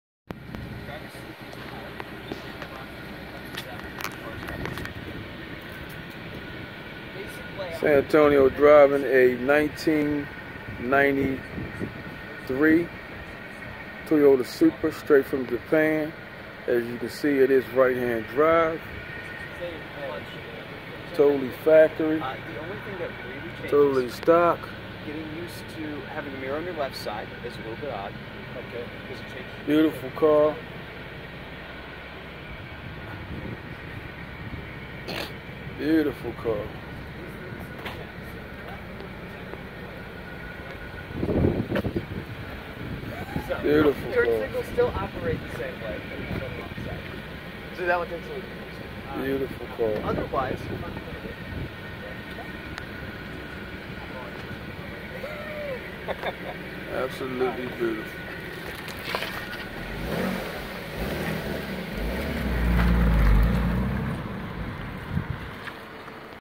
1993 Toyota Supra twin turbo
Test driving a right hand drive supra from Japanese classics. Driving right hand drive is fun. Wasn't hard to just drive it regular with the. The turn signals threw me off lol they're on the opposite side of the steering wheel and upside down. Fun experience.